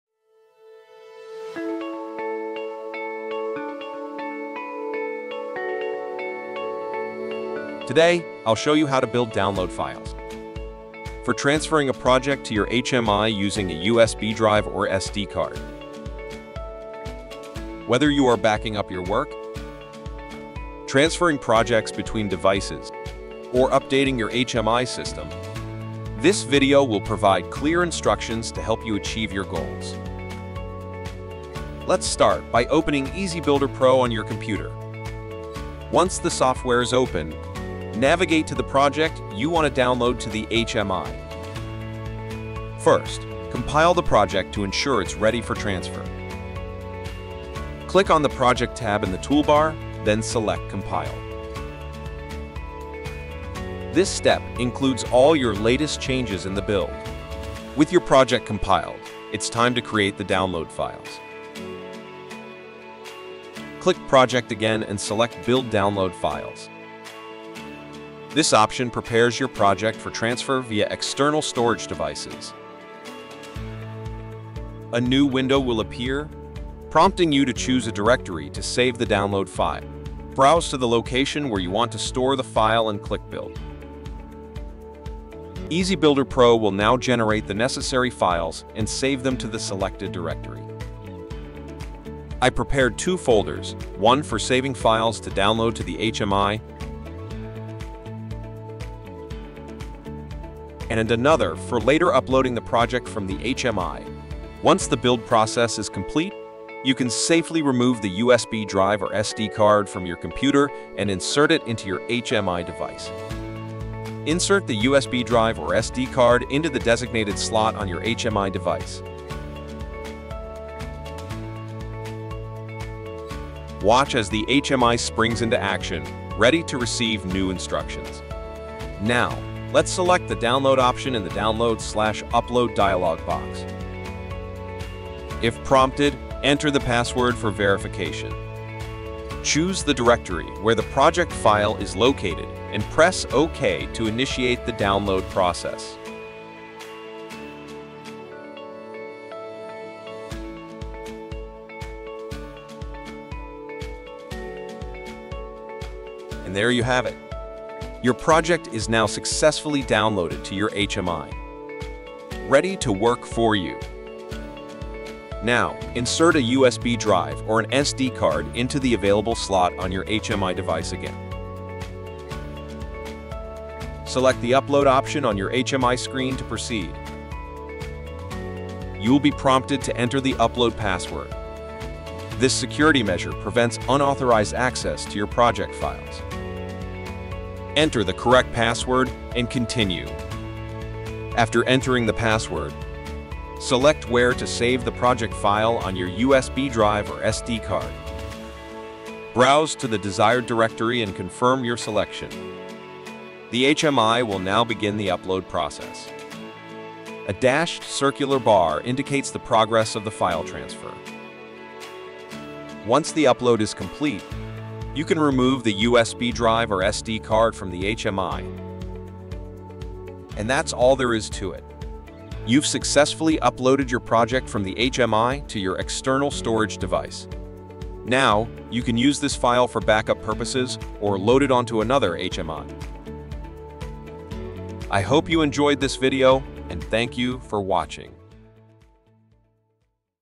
EBPro HMI Software Training: Download & Upload a Project via USB or SD
In this video you will learn how to prepare the download data for a USB Drive and an SD Drive, downloading to the HMI, as well as uploading a project from an HMI to the USB Drive or SD Card.

0:00 Introduction
0:31 Preparing the Download Data for USB Drive/SD Card
1:53 Download to HMI
2:45 Uploading a Project from HMI to USB Drive/SD Card